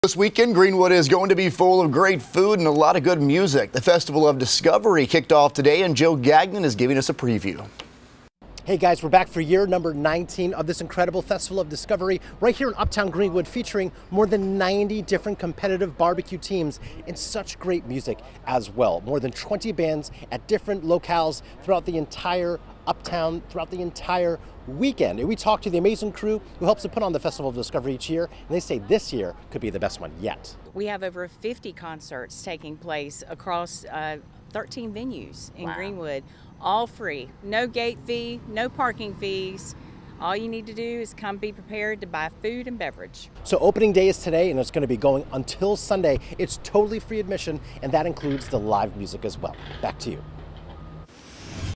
Festival of Discovery returns to Greenwood
Joe Gagnon previews the annual bbq and music festival, which will be underway through Sunday. (July 11, 2019)